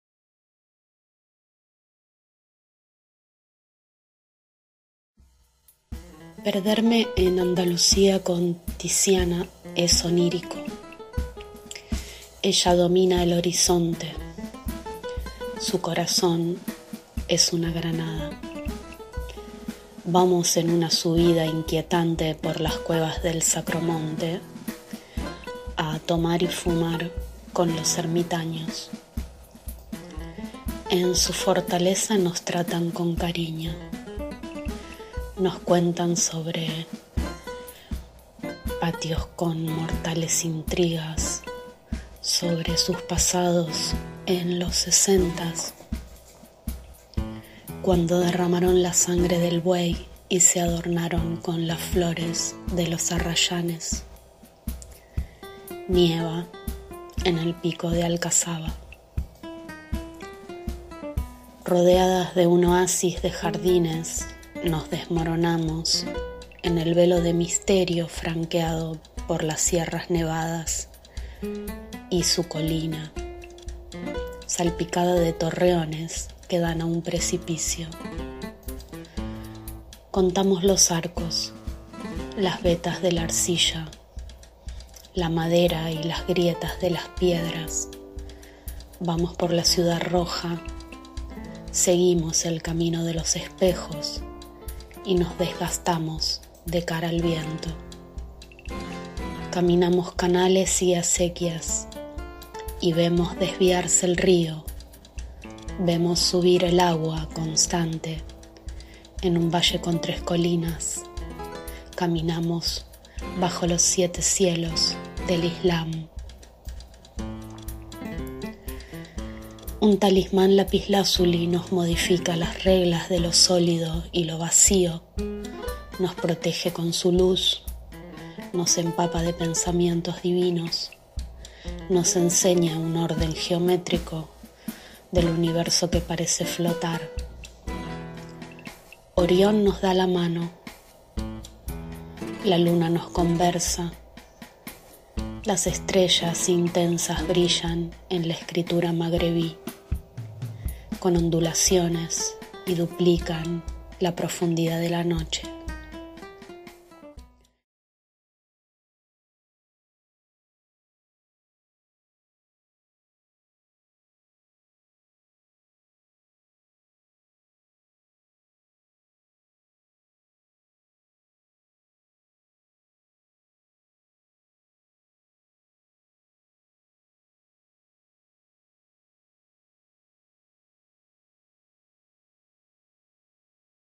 Magnolias Portraits, by Monica Weissel
An art therapy project. Portrait of Maria Fernanda De Broussais as part of Magnolias 2020